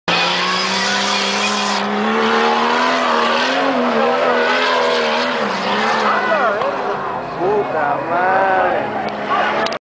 Sunday Nights in San Luis R.C.
Underground 1/8 mile 4x4 off-road drag races Sunday night on the Colorado River bed on the Mexican side. Notice there's no water, lol, the gringos don't let the water come through!